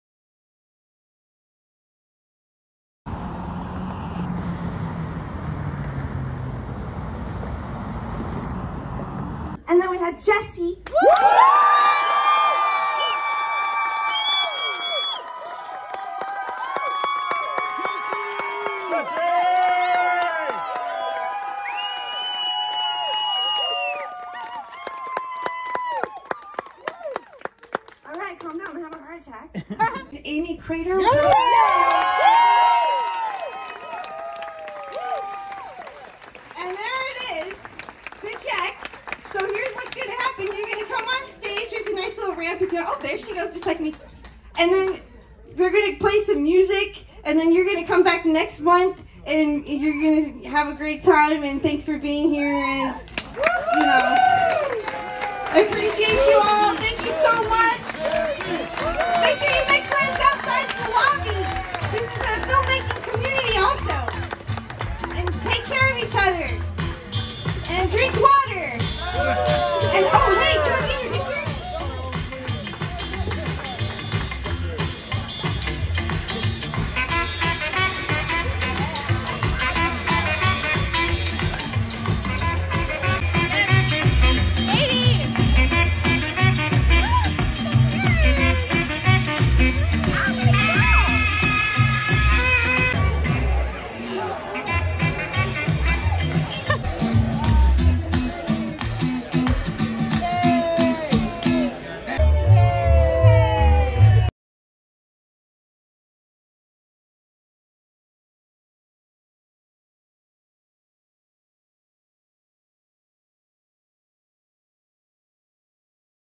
First Friday Shorts Winner August 2023
Amy Crater, Courtney Harmon, Katie Burkholder, Jen Mead, and Corbyn Mooney accept an award for Jessie at The Loft Cinema’s First Friday Shorts in August of 2023.